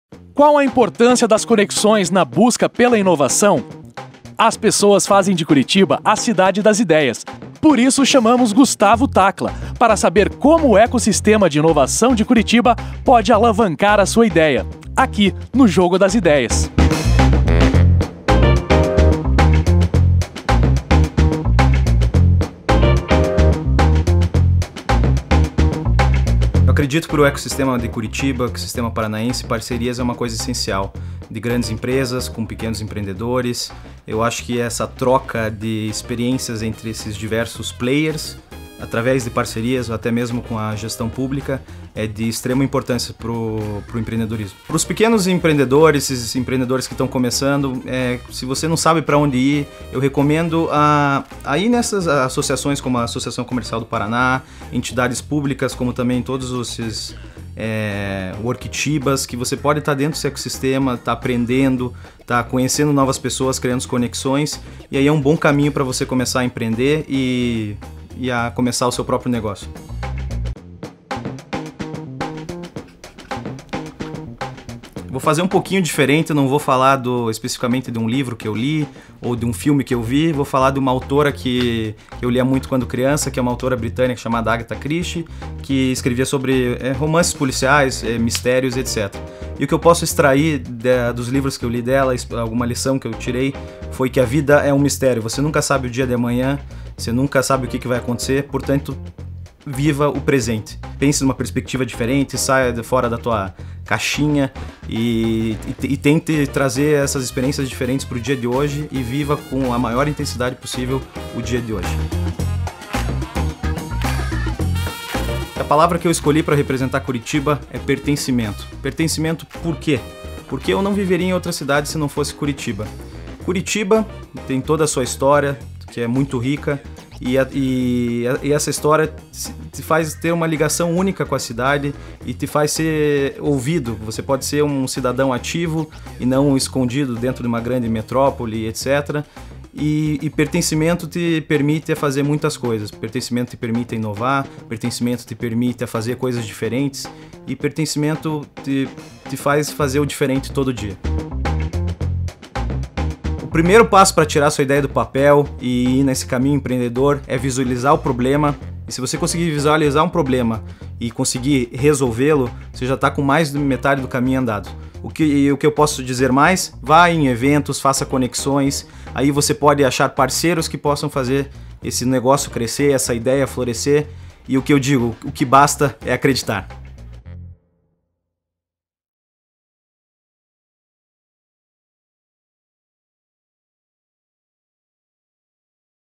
VAICURITIBA - Gustavo Tacla
Qual a importância das conexões na busca pela inovação?
Chamamos Gustavo Tacla para saber como o ecossistema de inovação de Curitiba pode alavancar a sua ideia. Para ele a parceria entre pequenos empreendedores e grandes empresas é de extrema importância para a evolução do seu negócio.
E você, tem saído da zona de conforto e criado boas conexões?